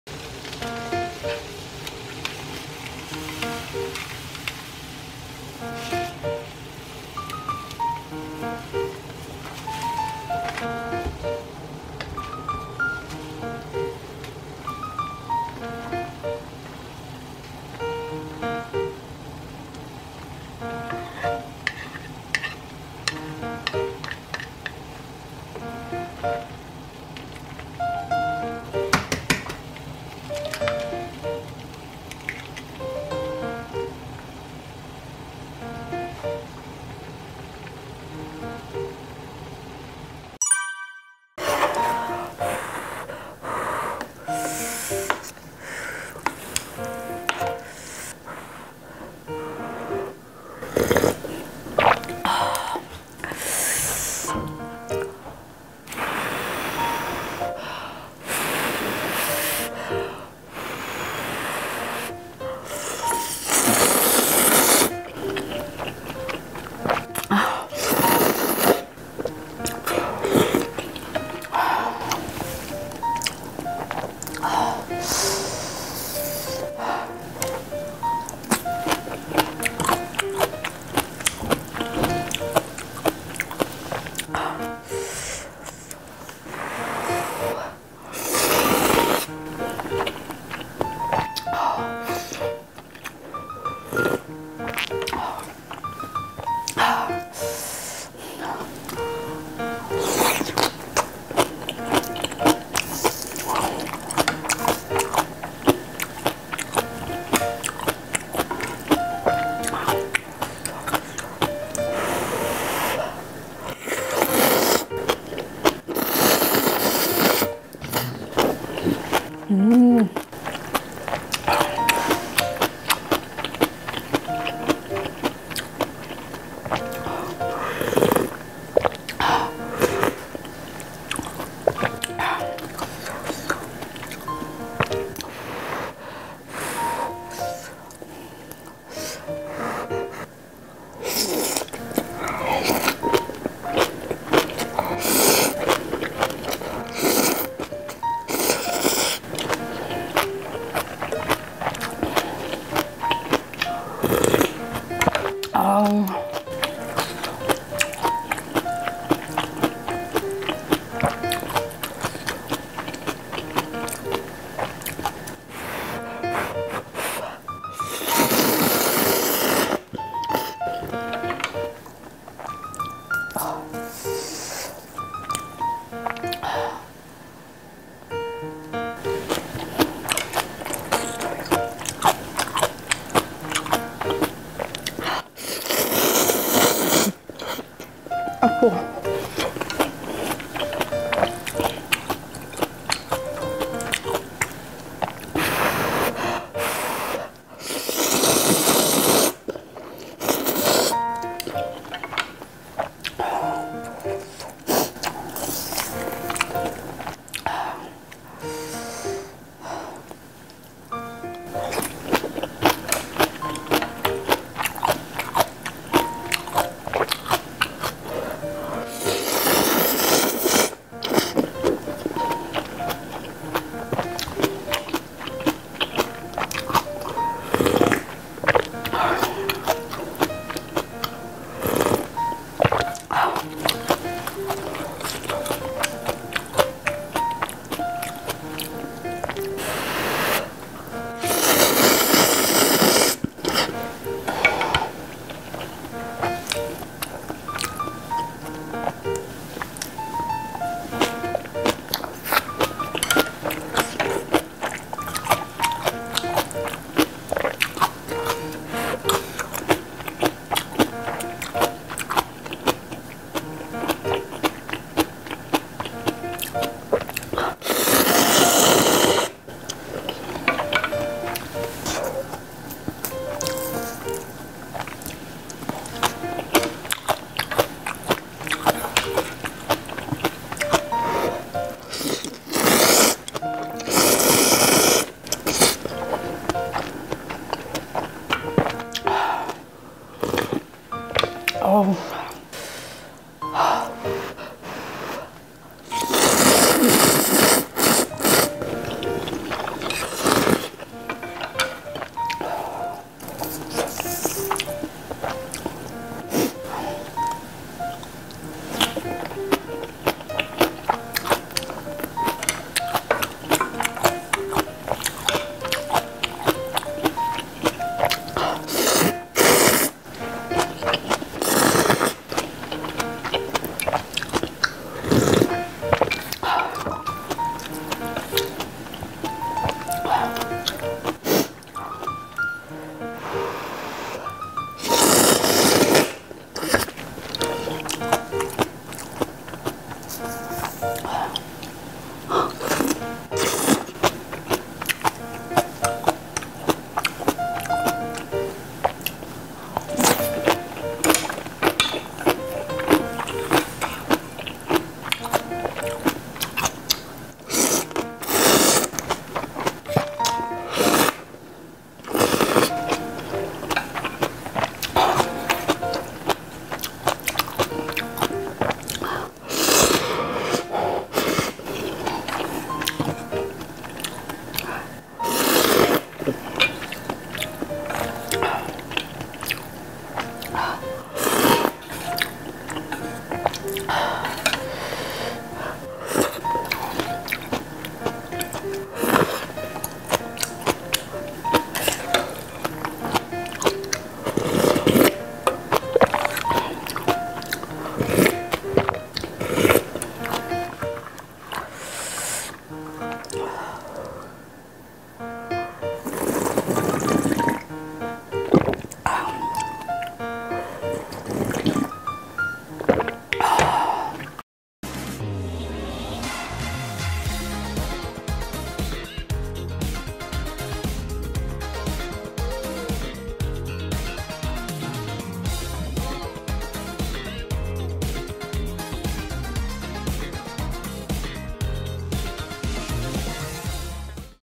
여러분... 라면 물 올리세요🍜🍜 ramyun MUKBANG
출출할땐 라면 한그릇 후루룩..🍜
A bowl of ramen is the best when you're hungry..🍜